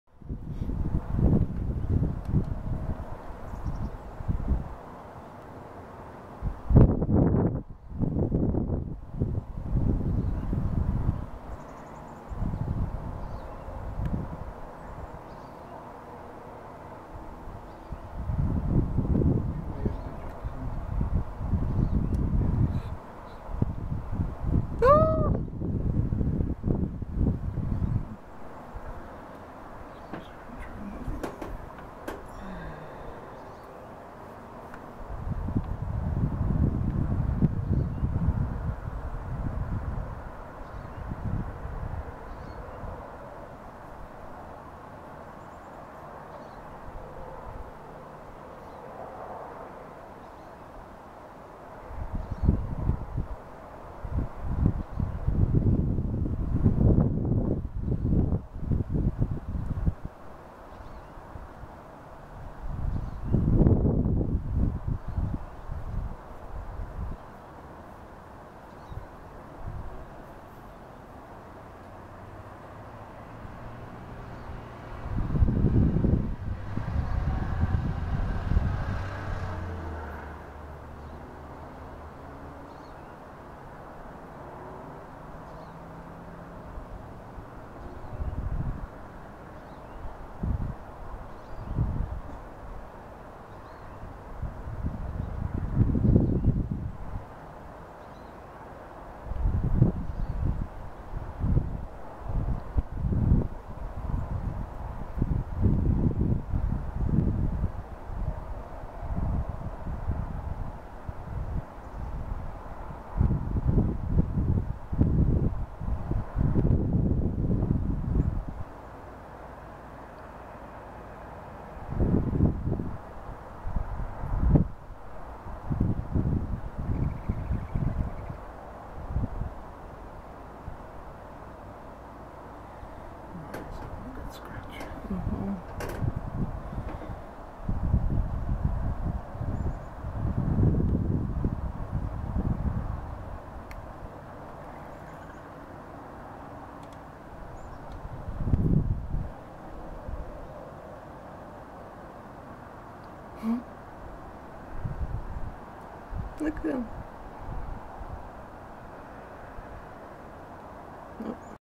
Fox hunting in snow
A healthy and spry young fox hunting mice, voles and whatever else on the hill behind our house. He stops in the middle to make breakfast from scratch... well, to itch his own, I say.